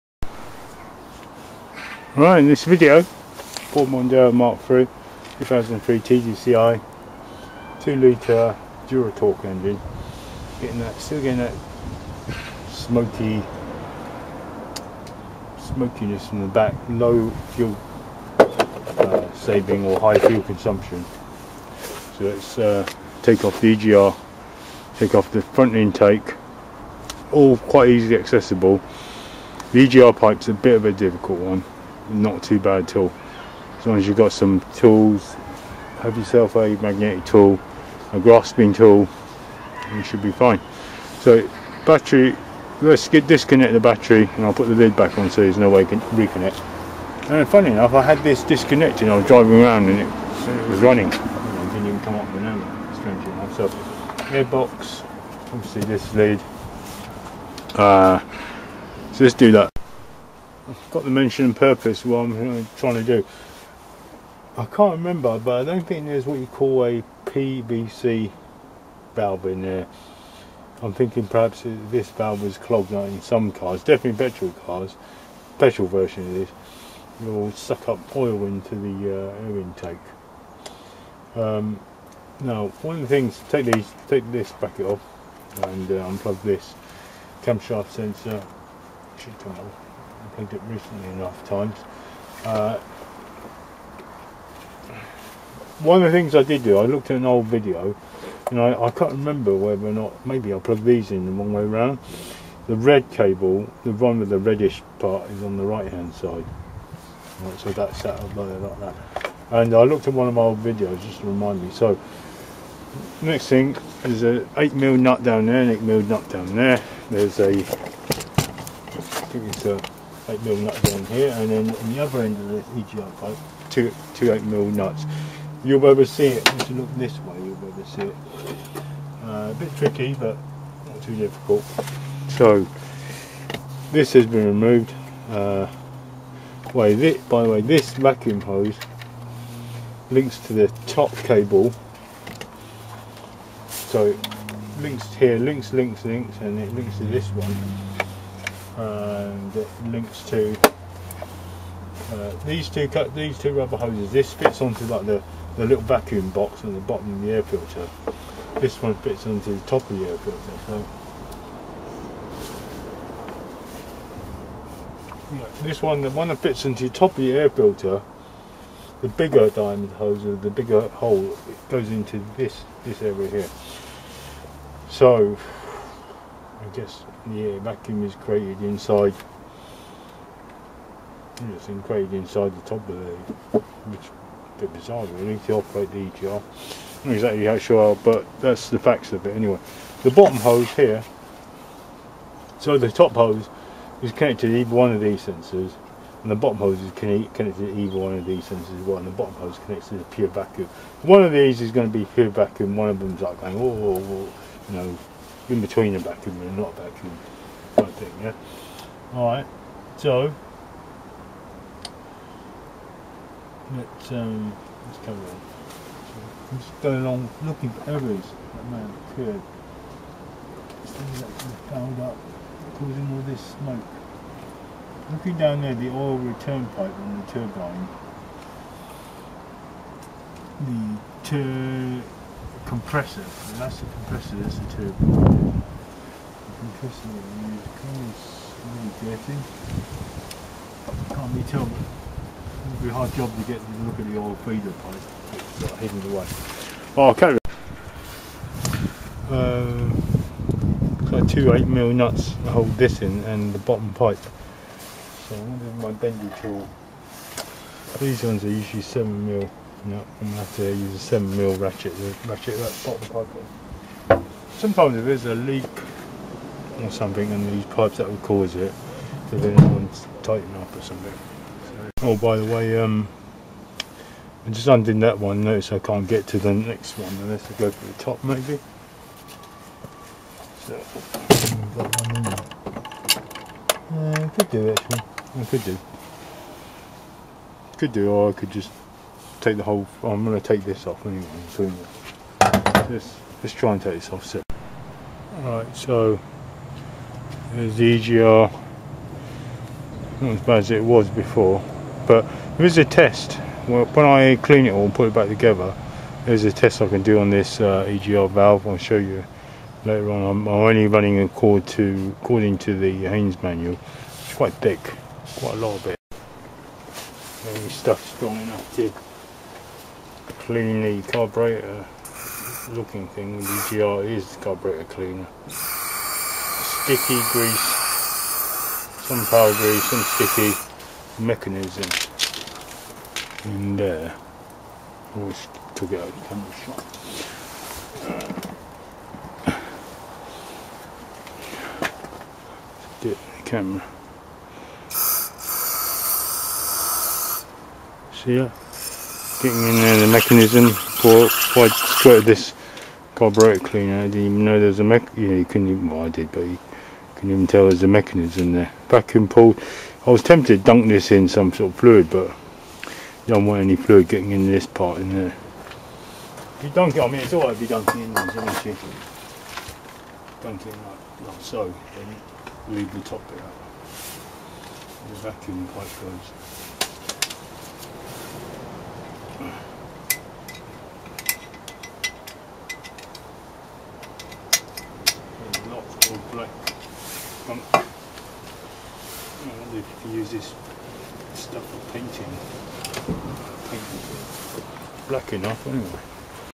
How to Remove and Clean the EGR on a Ford Mondeo 2003
Ford Mondeo mk3...the Exhaust Gas Recirculation Valve helps to reduce polluting NOx by recirculating exhaust gases to reduce combustion temperatures.

Amazon: Wynns 23381 Diesel EGR and Air Intake Cleaner, MAF, 150ml...about £9